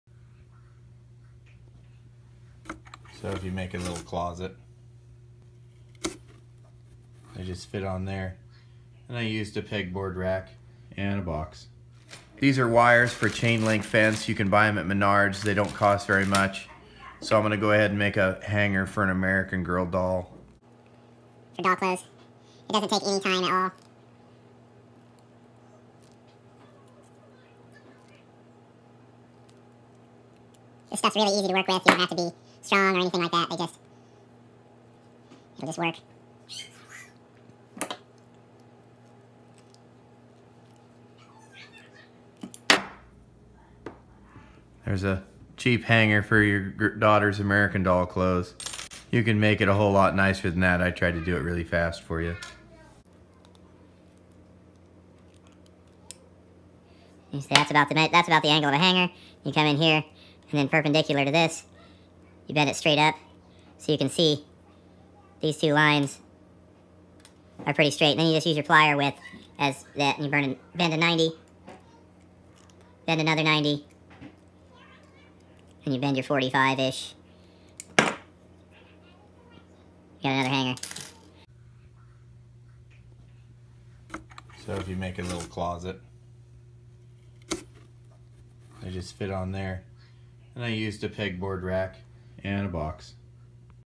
DIY Cheap American Doll Clothes Hanger
Sorry for the off topic video folks lol.  I figured if I could help someone make a kid happy for a couple bucks I would post it. I’m a girl dad so... making a closet for doll clothes got added to the daddy-do list.  Boom.  Under 3 minutes... I should apply for a nascar pit crew.